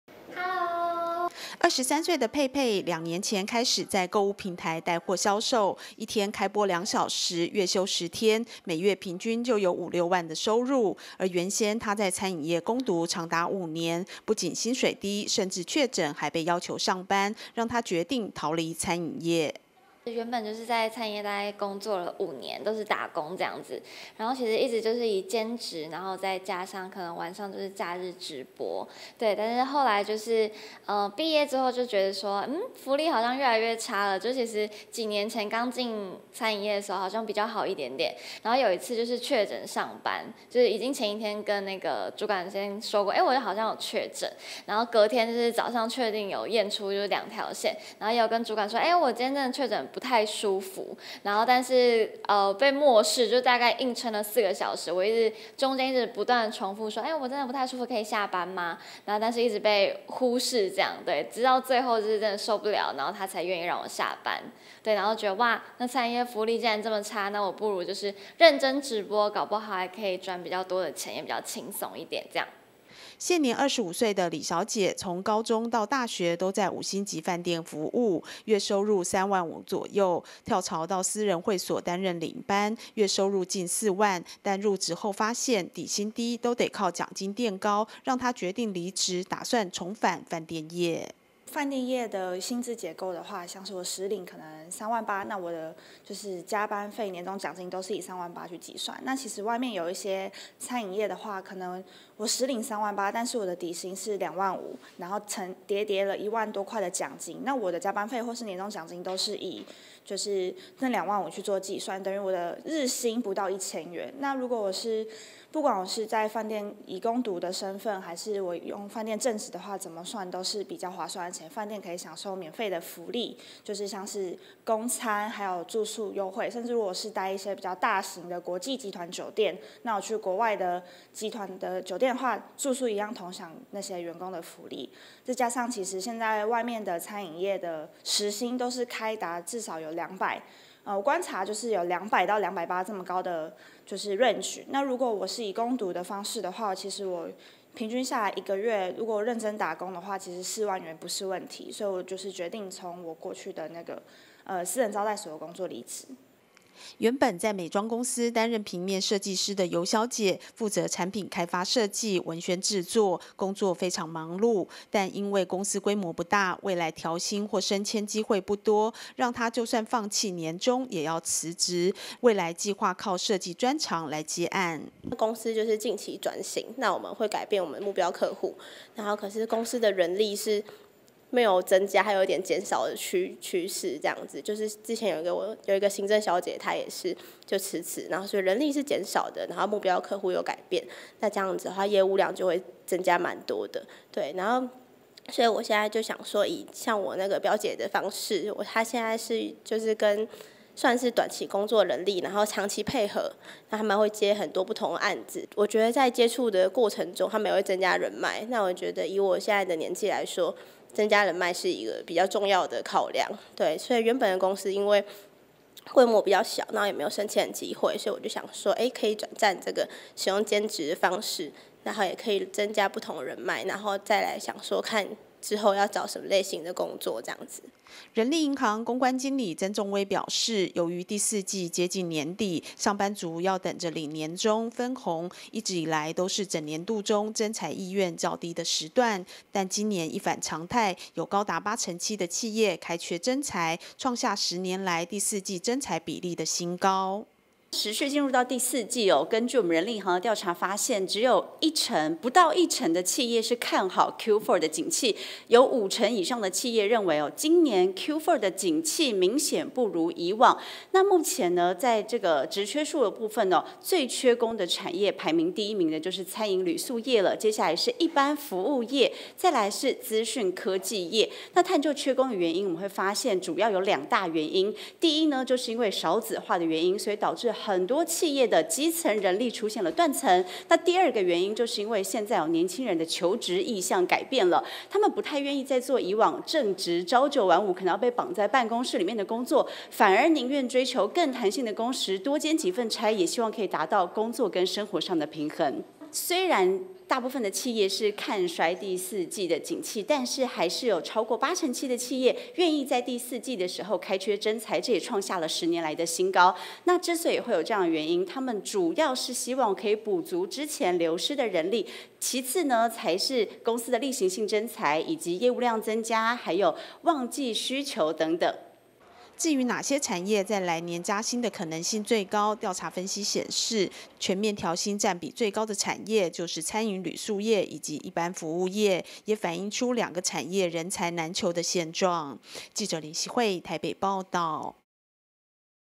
徵才 職缺|8成7企業第4季開缺徵才 這行業缺最兇
疫情之後內需商機爆發，不少行業都面臨缺工，根據人力銀行調查發現，多數企業對於第四季的業績仍保守，有近5成企業認為，景氣比去年同期差，只有6.1%看好景氣。儘管企業看淡景氣，卻絲毫沒有衝擊到徵才意願，高達8成7企業在第4季開缺徵才，創下10年來同期新高。

23歲的沛沛，兩年前開始在購物平台帶貨銷售，一天開播2小時，月休十天，每月平均就有5-6萬收入，而原先她在餐飲業工讀長達5年，不僅薪水低，甚至確診還被要求上班，讓她決定逃離餐飲業。

現年25歲的李小姐，從高中到大學都在五星級飯店服務，月收入3萬5左右，跳槽至私人會所擔任領班，月收近4萬，但入職後發現，底薪低都得靠獎金墊高，讓她決定離職，打算重返飯店業。

原本在美妝公司擔任平面設計師的游小姐，負責產品開發設計、文宣製作，工作非常忙碌，但因為公司規模不大，未來調薪或升遷機會不多，讓她就算放棄年終也要辭職，未來計畫靠設計專長來接案。

人力銀行公關經理曾仲葳表示，由於第4季接近年底，上班族要等著領年終、分紅，一直以來都是整年度中，徵才意願較低的時段，但今年一反常態，有高達8成7的企業開缺徵才，創下10年來第4季徵才比例的新高。

至於哪些產業在來年加薪的可能性最高，調查分析顯示，全面調薪占比最高的產業，就是餐飲旅宿業以及一般服務業，也反映出兩個產業人才難求的現況。